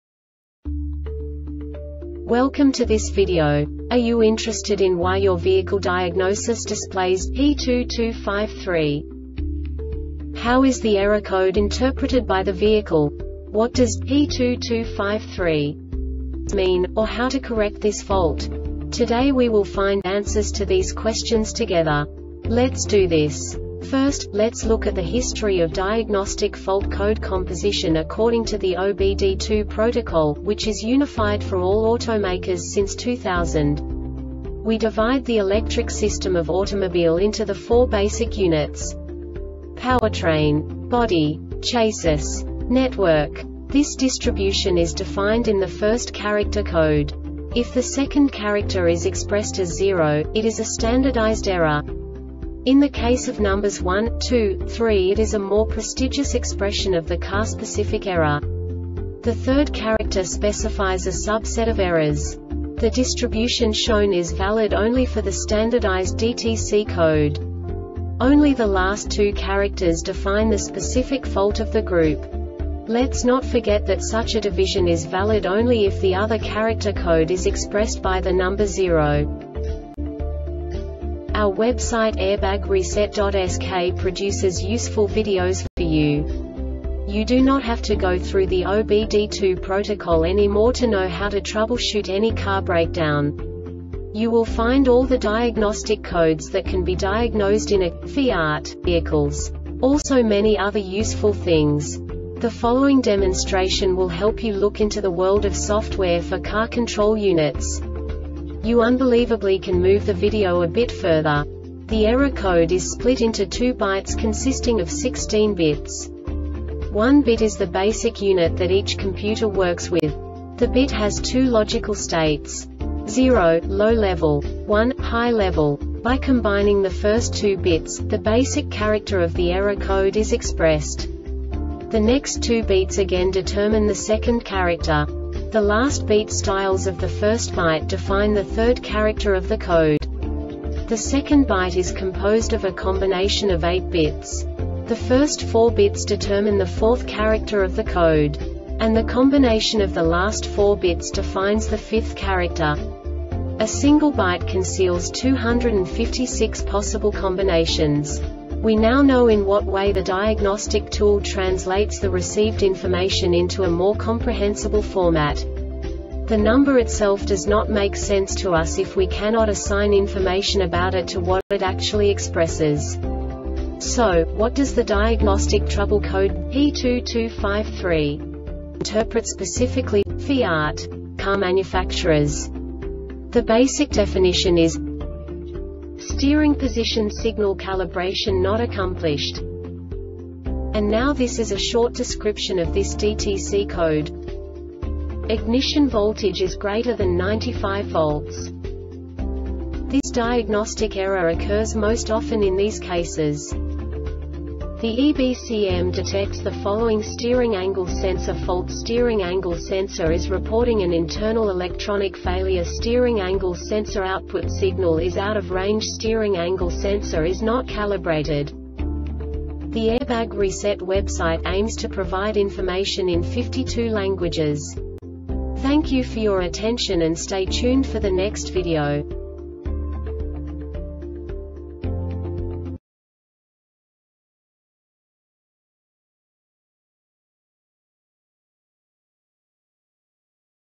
DTC Fiat P2253 Short Explanation
Contents:
0:21 Basic DTC analysis according to OBD2 protocol standard.
1:48 Insight into programming
2:58 A diagnostically understandable description of the Diagnostic Trouble Code
3:05 Basic error definition

Make:
FIAT
Code:
P2253
Definition:
Steering Position Signal Calibration Not Accomplished
Description:
Ignition voltage is greater than 9.5 volts.
Cause:
The EBCM detects the following steering angle sensor fault:Steering angle sensor is reporting an internal electronic failure.Steering angle sensor output signal is out of range.Steering angle sensor is not calibrated.